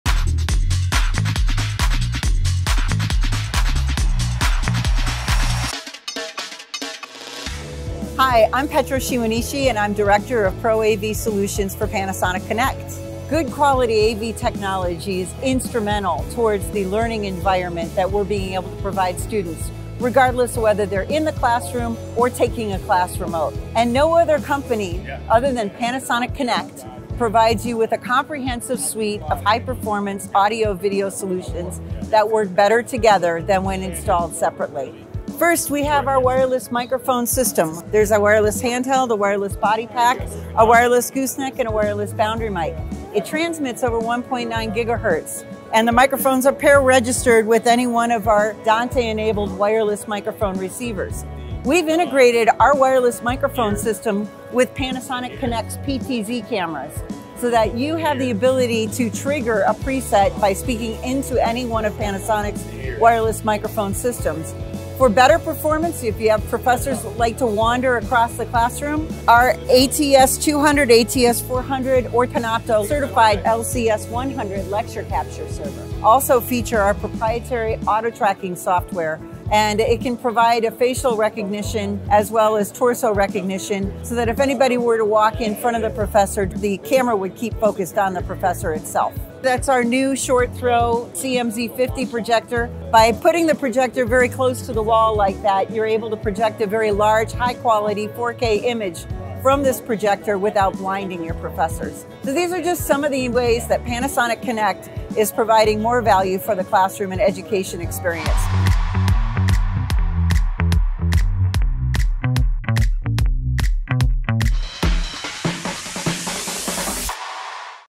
Building a Connected Classroom
A connected classroom leverages an ecosystem of audio, video, projection and display technology to change the way teachers and students interact for the better. In this video, Petro Shimonishi, Director of Solutions Development, discusses Panasonic Connect's latest audiovisual technology that increases collaboration for engaging, productive learning experiences.

To learn more about Panasonic Connect's solutions for Education, head to our